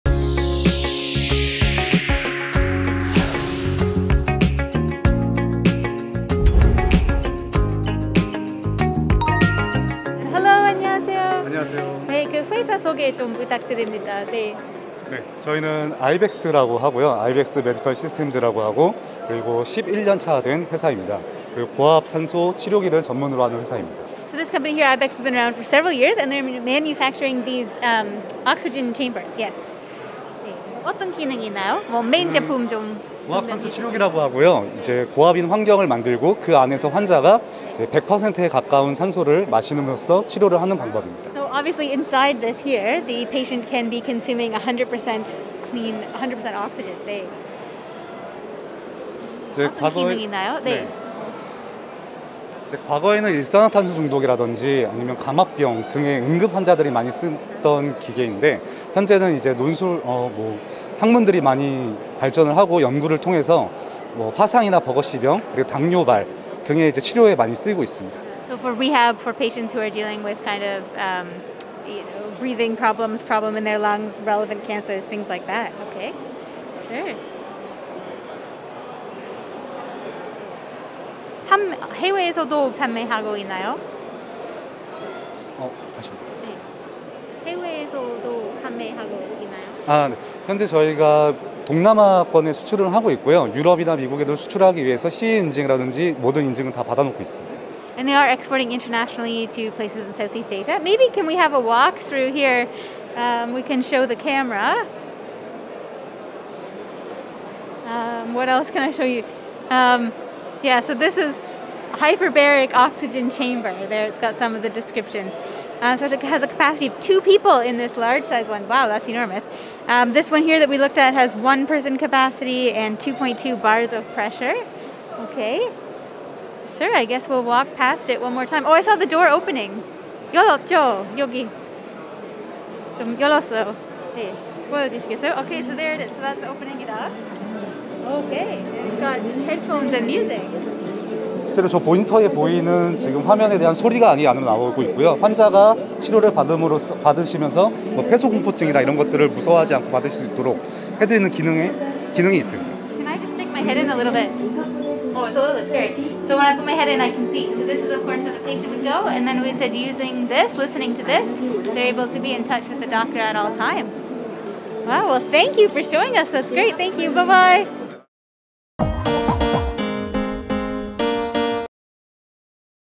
IBEX MEDICAL SYSTEMS - GMES 2022 (강원의료기기전시회)
GMES 2022 강원의료기기전시회에서 진행된 아이벡스 현장 부스 및 제품 소개 인터뷰입니다.